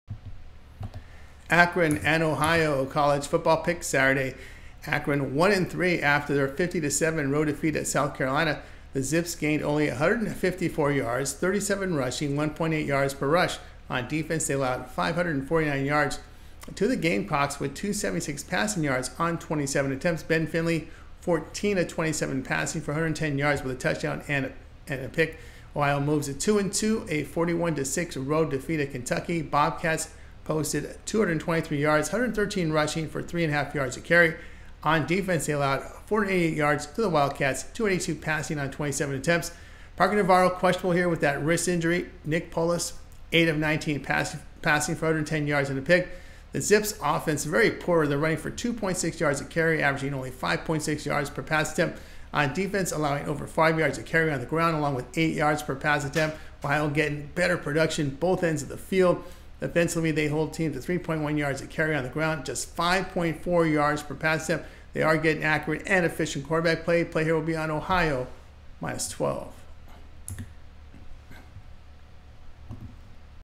Akron vs. Ohio Pick 9/28/24 NCAAF Week 5 Predictions
9/28/24 Akron vs. Ohio NCAAF Picks & Prediction by Tony Tellez,